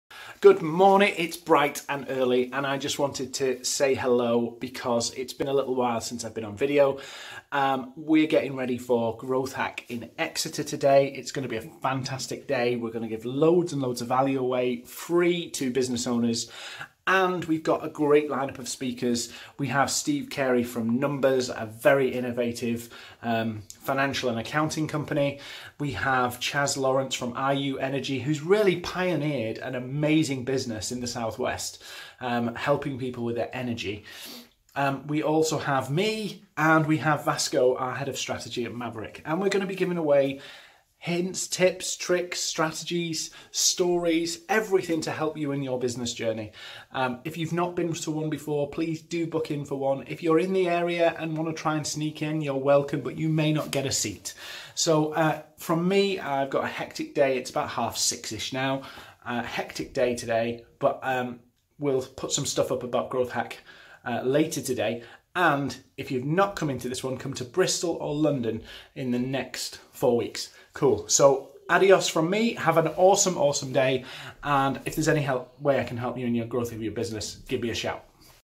Growth Hack Exeter
Really excited for today!

We are gonna be sharing...

- Start up survival stories
- Building for your future
- Lessons learnt by global brands

Plus a ton of stuff that will help you practically grow your business and win new customers.

Exeter here we come, if you're booked in see you there...

If you haven't got your free ticket, you can turn up on spec but you may have to stand.

2pm - Exeter - Sandy Park - Free Entry

Guest Speakers...
Steve Carey & Chaz Lawrence